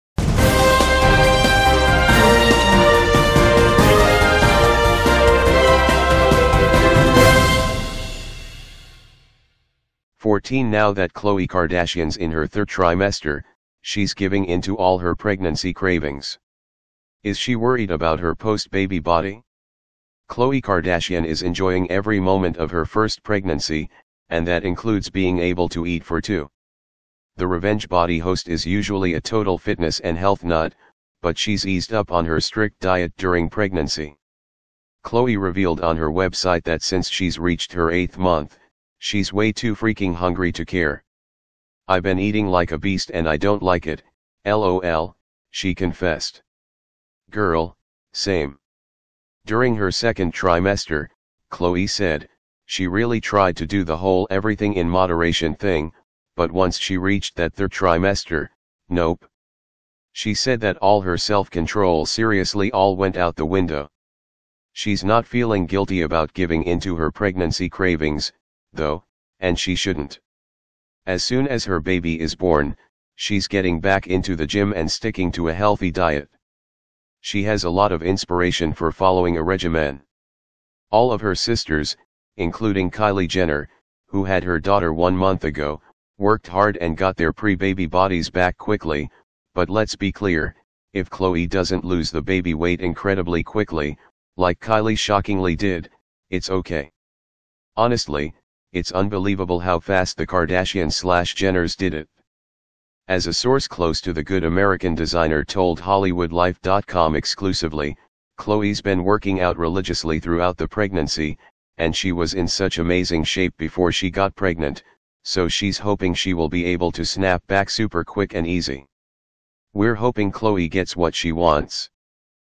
Khloe Kardashian Says She’s ‘Eating Like A Beast’ While Pregnant Will She Bounce Back Like Kylie
✅ USA BREAKING NEWS TODAY: Khloe Kardashian Says She’s ‘Eating Like A Beast’ While Pregnant Will She Bounce Back Like Kylie
✅ TOGETHER WE WILL MAKE AMERICA GREAT AGAIN!
► Source/Credit/Read more:Now that Khloe Kardashian’s in her 3rd trimester, she’s giving into all her pregnancy cravings! Is she worried about her post-baby body?
Khloe Kardashian is enjoying every moment of her first pregnancy, and that includes being able to eat for two! The Revenge Body host is usually a total fitness and health nut, but she’s eased up on her strict diet during pregnancy. Khloe revealed on her website that since she’s reached her eighth month, she’s way too freaking hungry to care! “I’ve been eating like a beast and I don’t like it, LOL,” she confessed. Girl, same.
During her second trimester, Khloe said, she really tried to do the whole “everything in moderation” thing, but once she reached that third trimester…nope! She said that all her self-control “seriously all went out the window.” She’s not feeling guilty about giving into her pregnancy cravings, though (and she shouldn’t). As soon as her baby is born, she’s getting back into the gym and sticking to a healthy diet! She has a lot of inspiration for following a regimen. All of her sisters, including Kylie Jenner, who had her daughter one month ago, worked hard and got their pre-baby bodies back quickly!But let’s be clear: if Khloe doesn’t lose the baby weight incredibly quickly, like Kylie shockingly did, it’s okay! Honestly, it’s unbelievable how fast the Kardashian/Jenners did it! As a source close to the Good American designer told HollywoodLife.com EXCLUSIVELY, “Khloe’s been working out religiously throughout the pregnancy, and she was in such amazing shape before she got pregnant, so she’s hoping she will be able to snap back super quick and easy.” We’re hoping Khloe gets what she wants!